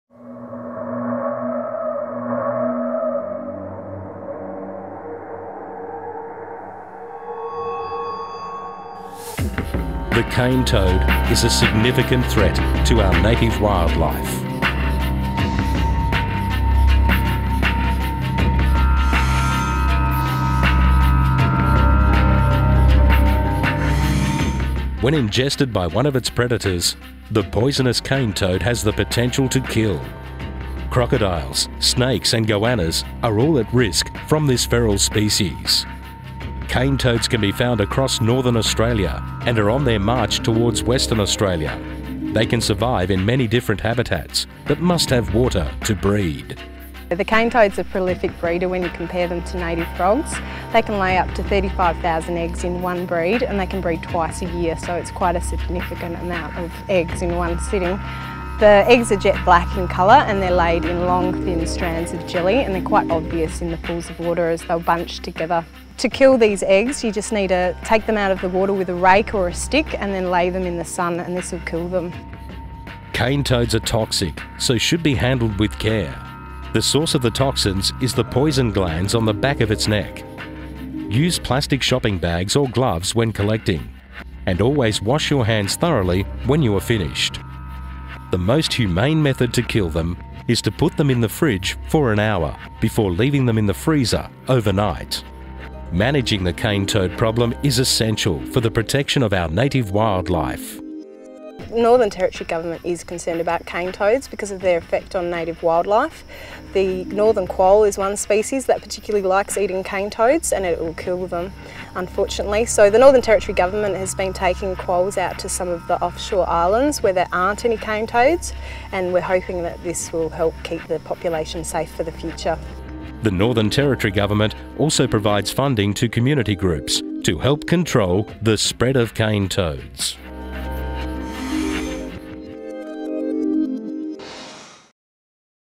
Cane Toad - Rhinella marina
Cane toads are a major pest in the Northern Territory (NT).

They are spreading throughout northern Australia and are a threat to native wildlife.

Cane toads are toxic if the toxin is ingested or rubbed into your eyes.

The source of the toxin is two large glands behind the eyes and on the back of the neck. The toxin is usually on the toad's skin, but can be sprayed lightly if the toad is handled roughly.